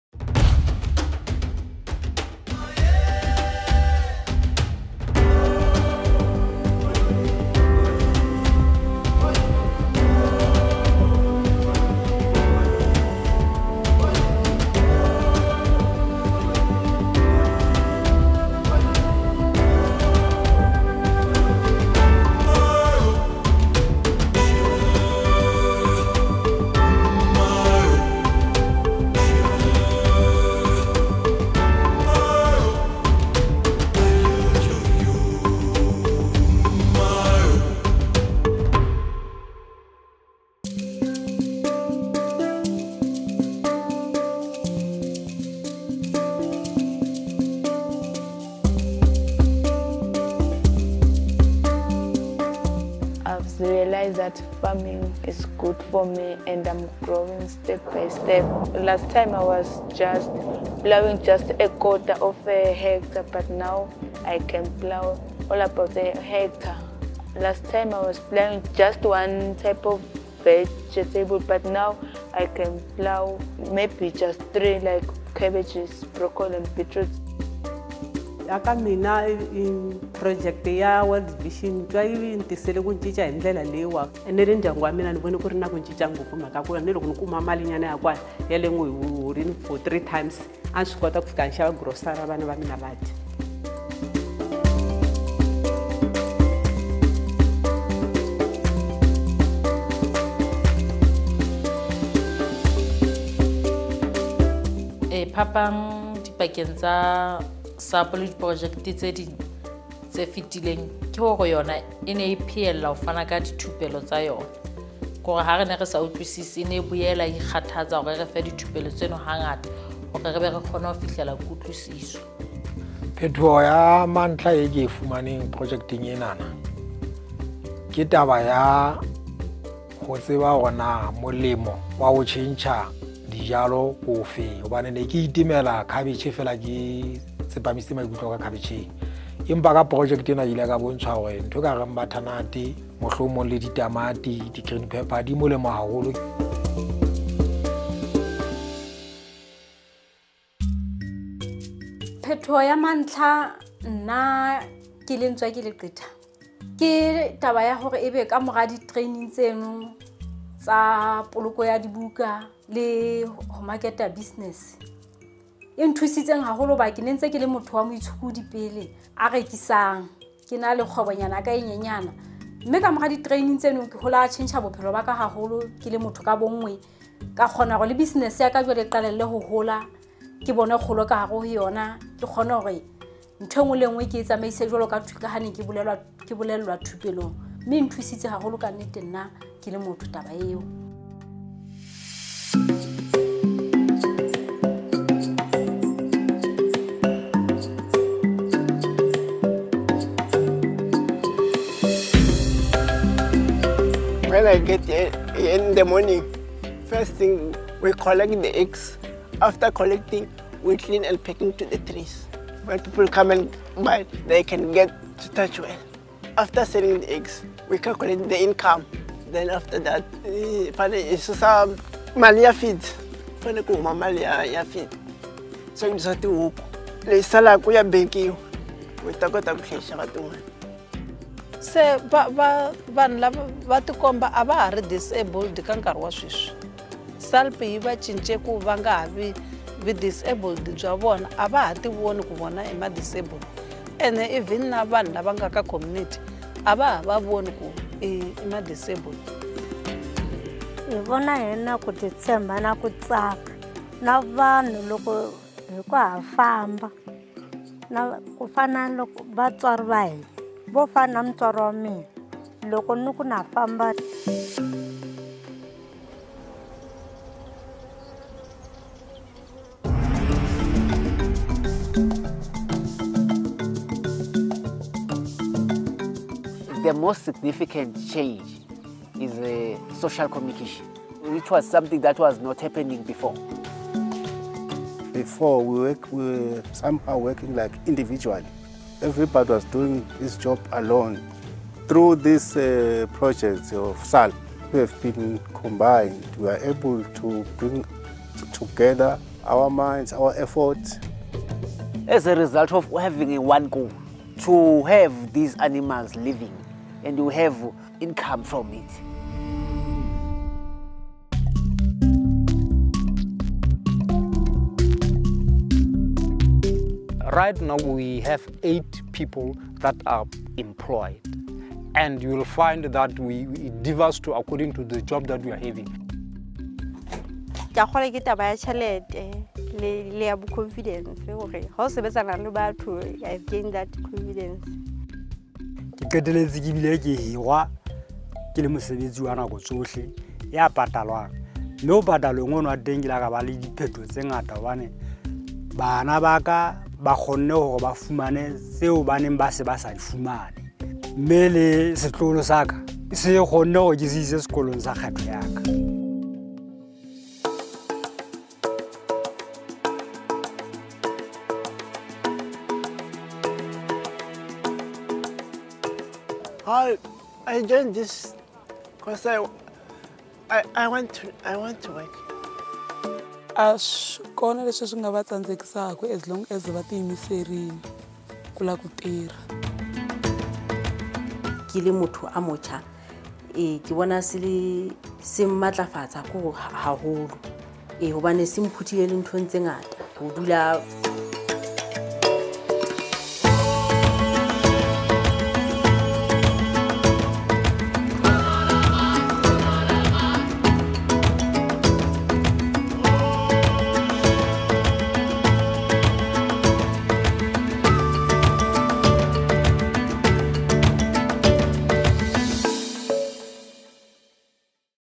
SALP stories of change
Hear stories of change from participants in World Vision's Southern Africa Livelihoods Project (SALP), supported by the Australian Government through the Australian NGO Cooperation Program. Find out how the project helped them to build sustainable small businesses and grow their skills and business knowledge.

SALP was implemented in South Africa, Lesotho and Eswatini from July 2017 to June 2020.